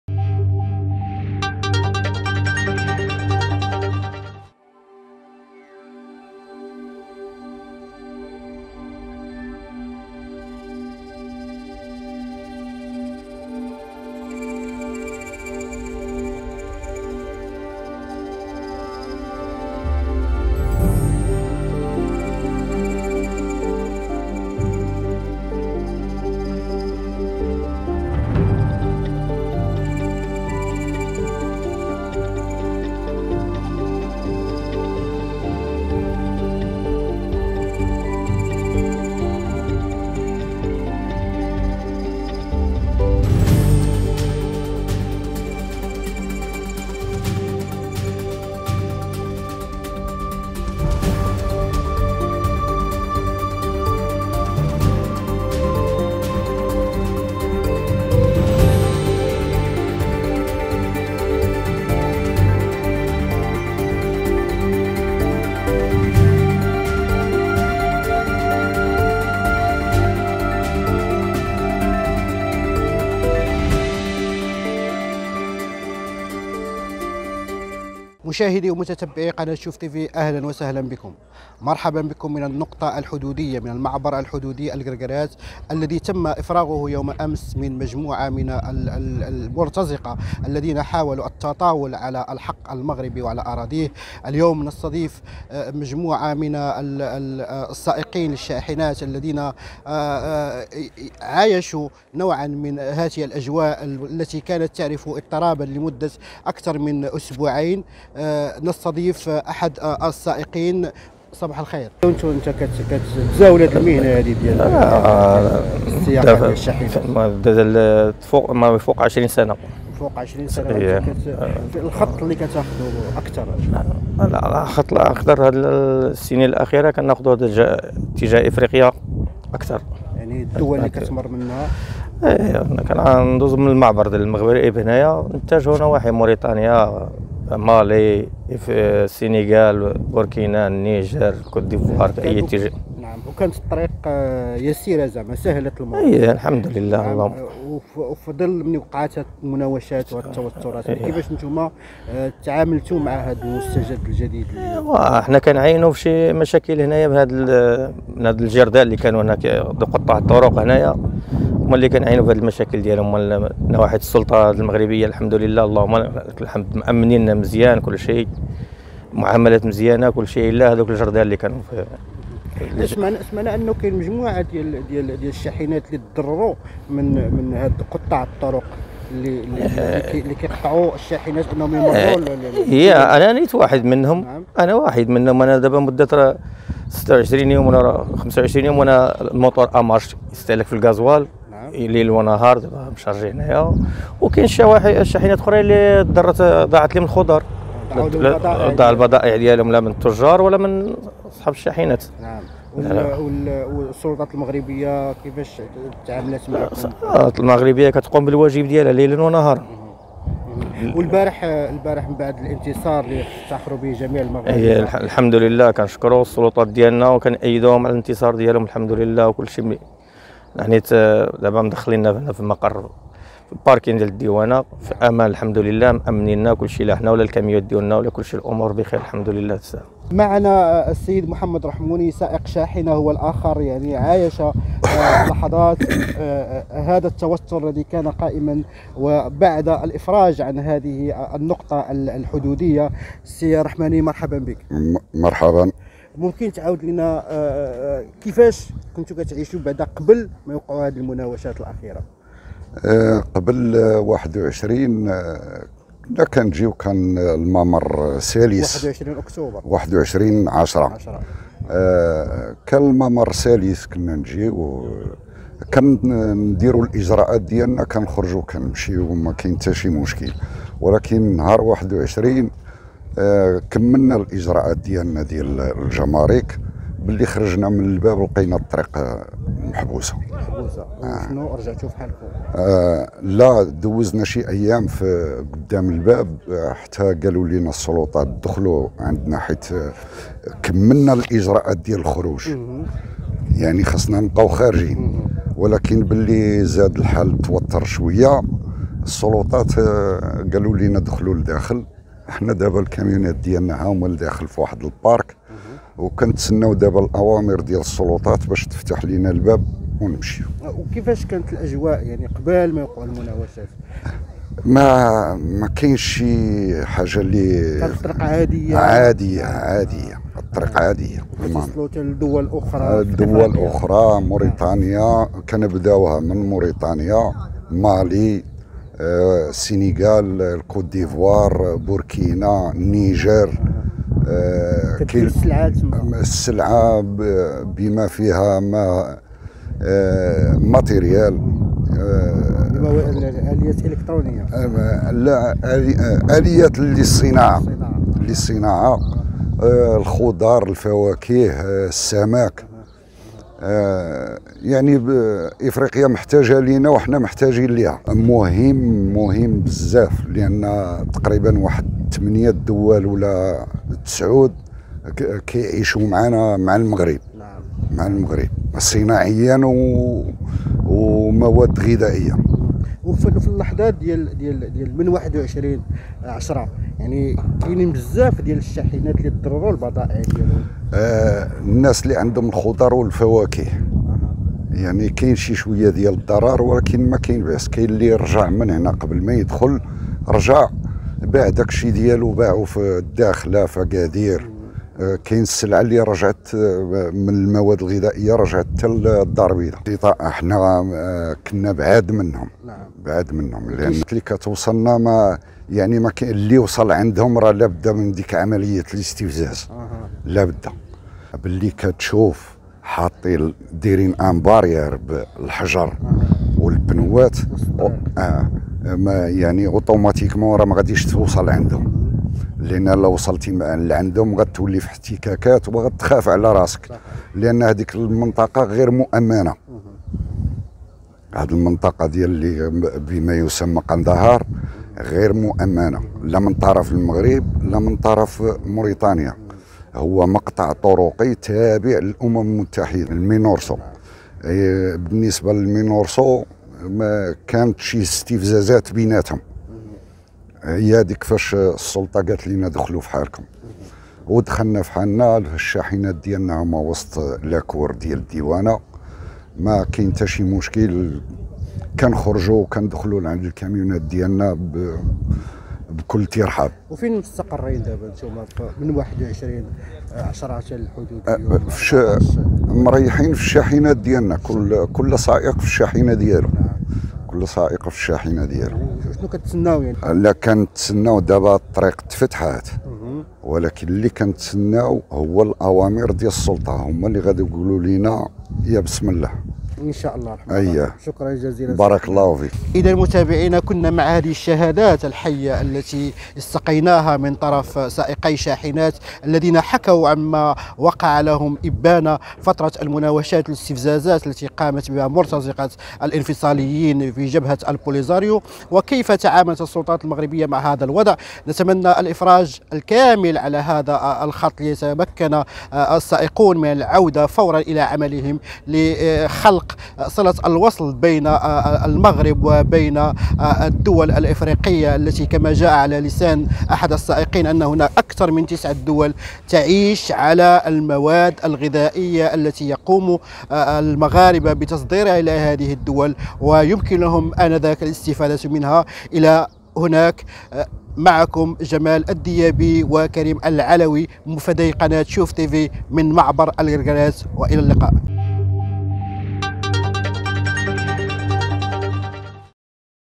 من قلب الكركرات..تصريحات قوية لشهود عيان يكشفون لحظة تدخل الجيش المغربي لسحق الانفصاليين
شوف تيفي، عاينت الوضع بمنطقة الكركرات، ساعات بعد تدخل الجيش المغربي لطرد مرتزقة البوليساريو.